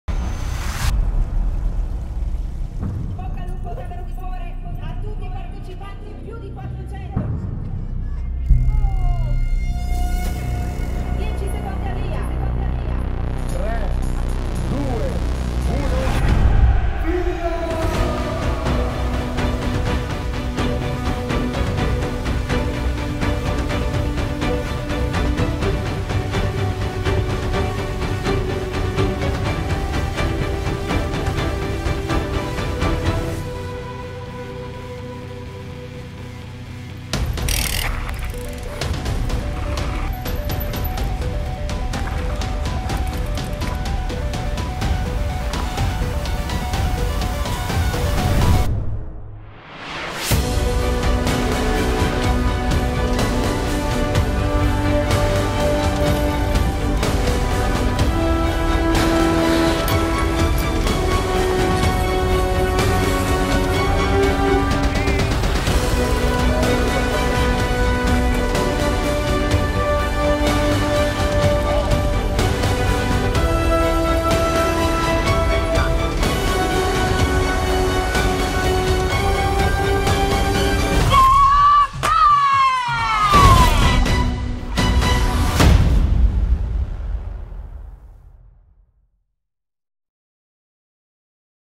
Monterosa Prestige mountain bike marathon
In Val d’Ayas, al cospetto del Monte Rosa, scopri lo spettacolare percorso della Monterosa Prestige, la mountain bike marathon della Valle d'Aosta.